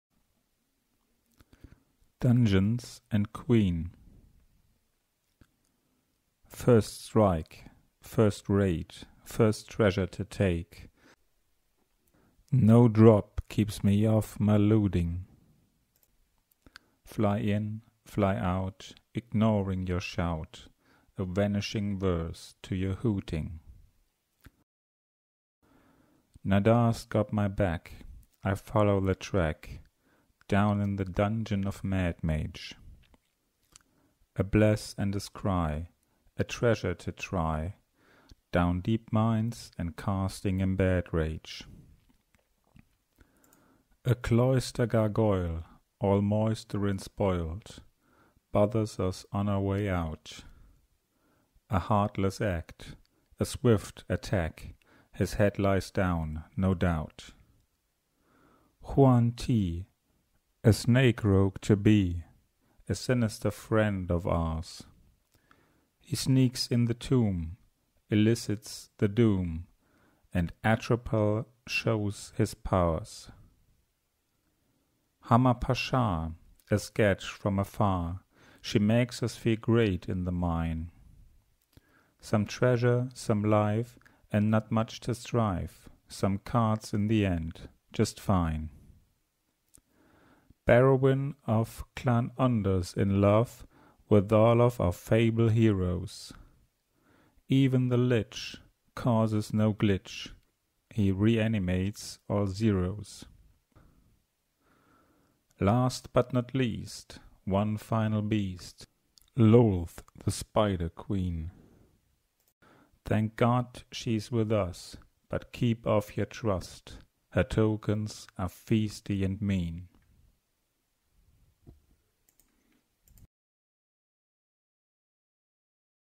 Dungeons and Queen
No 30 - Behold the ultimate dungeon crawl!

This poem is about the Esper Dungeon deck by LegenVD I know, I know, his video is two weeks old now, but I can assure you that this is one of my best poems so far :)

Sincerely,
the MTGPoet